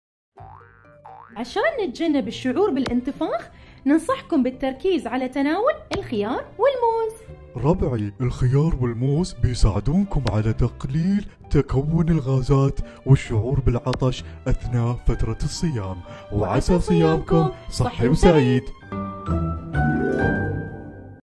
الشعور بالإنتفاخ | تمرة وتمر
لتجنب الشعور بالانتفاخ  ينصح بالتركيز على تناول الخيار والموز، اللذان يساعدان على تقليل تكون الغازات و الشعور بالعطش أثناء فترة الصيام